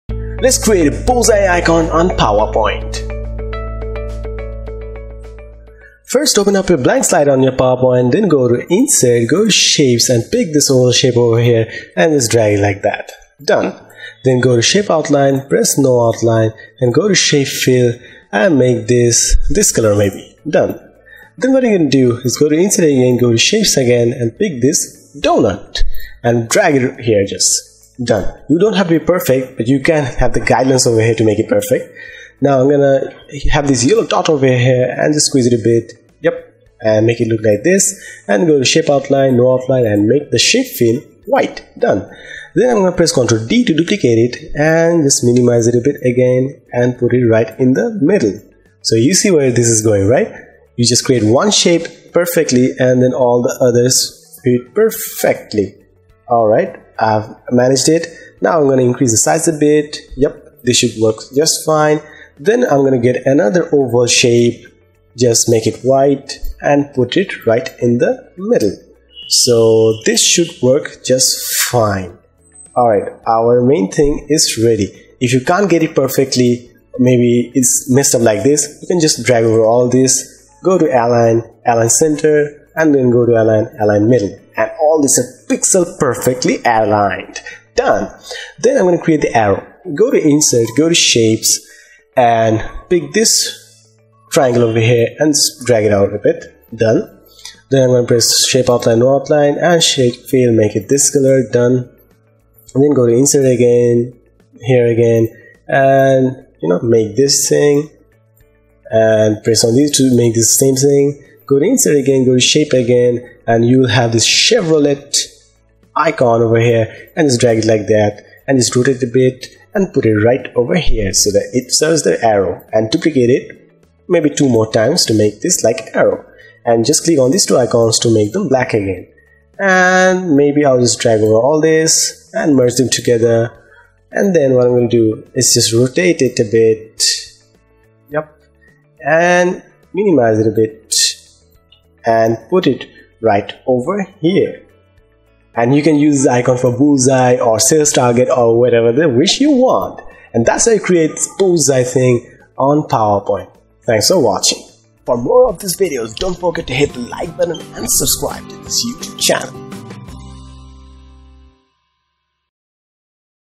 Bullseye! Advanced PowerPoint Icon making! (Microsoft PowerPoint 2013) | PowerPoint Pro
Powerpoint can be an awesome tool for vector arts! Hope this video ignites creative inspiration: D
You can also check out some of my slide on slideshare!
Follow through this simple video and learn how easy it is to create super cool and simple icon in a blink of an eye.
Powerpoint 2013 was used making this video!
You may suggest any icon or animation you want me to make a tutorial on!